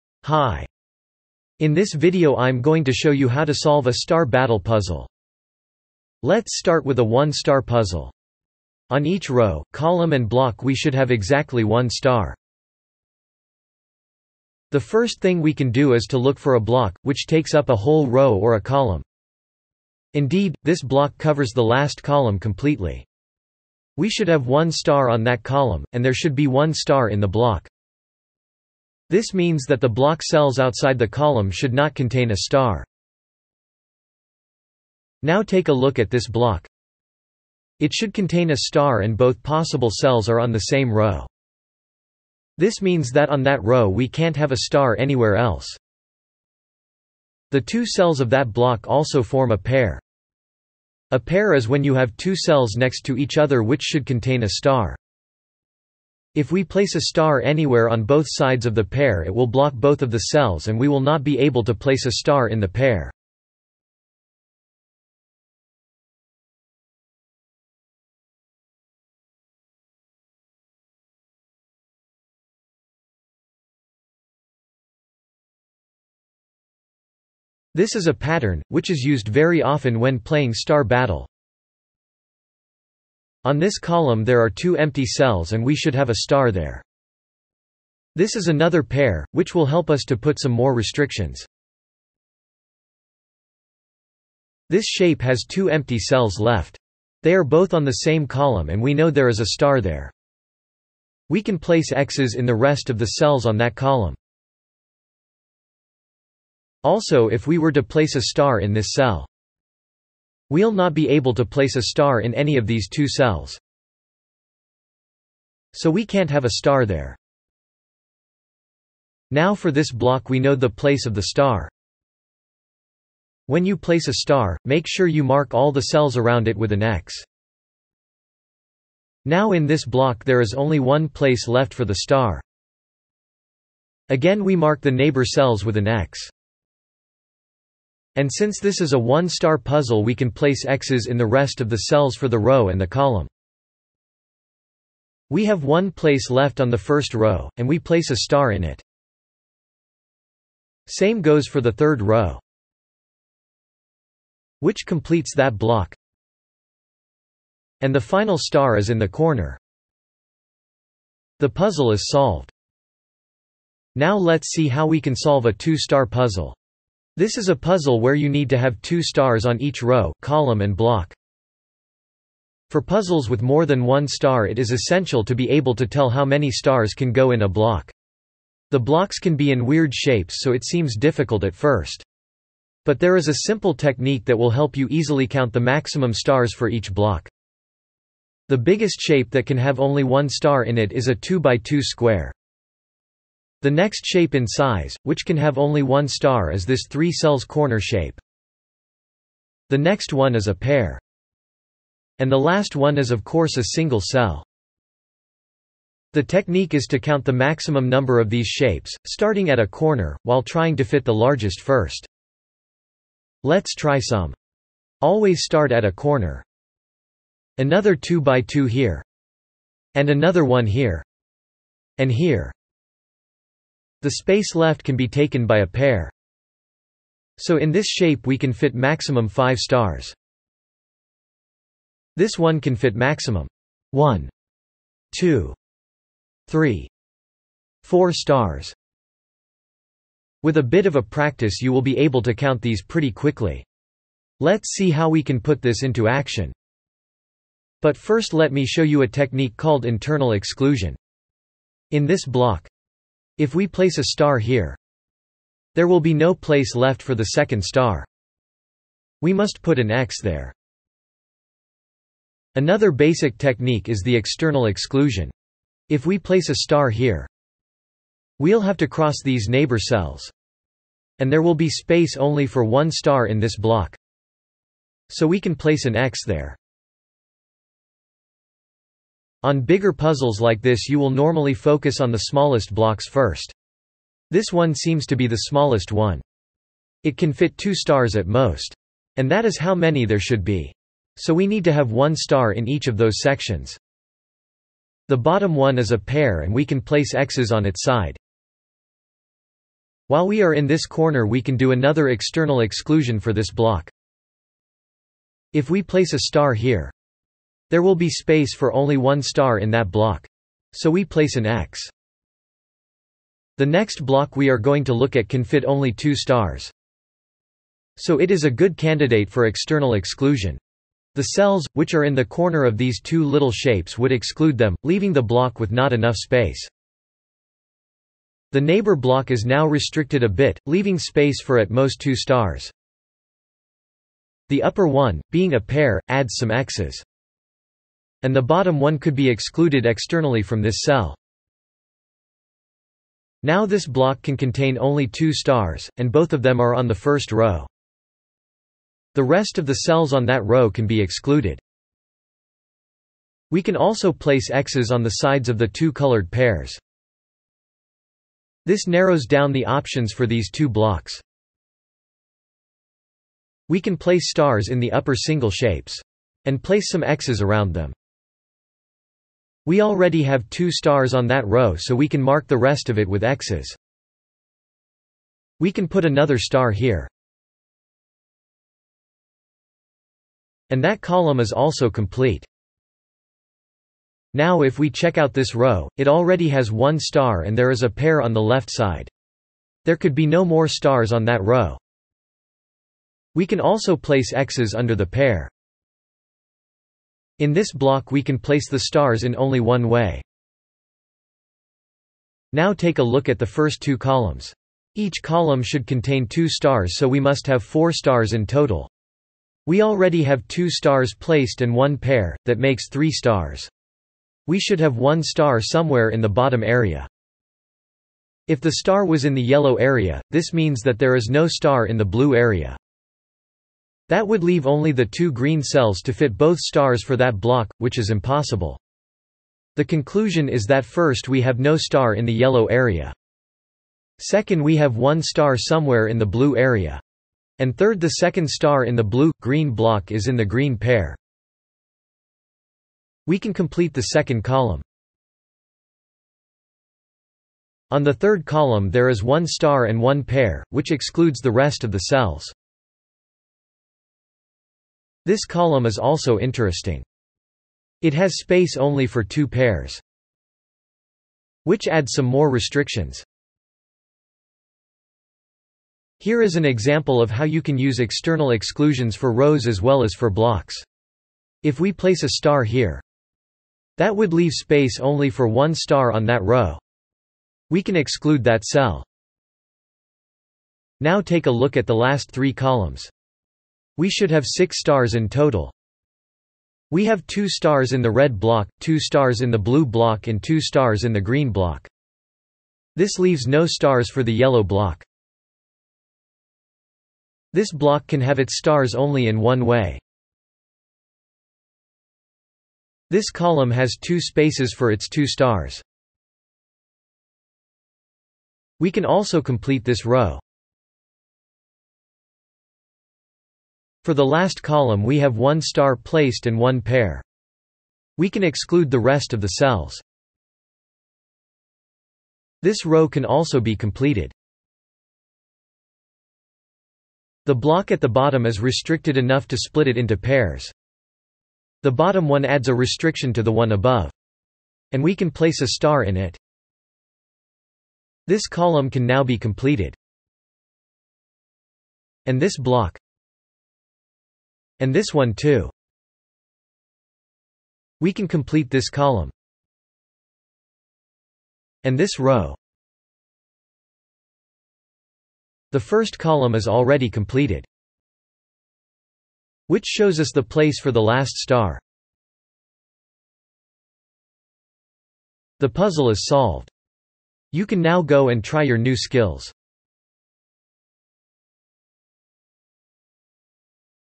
Star Battle Tutorial - #2 Solving a Puzzle
Solving a Star Battle puzzle.

You can find tons of puzzles available to solve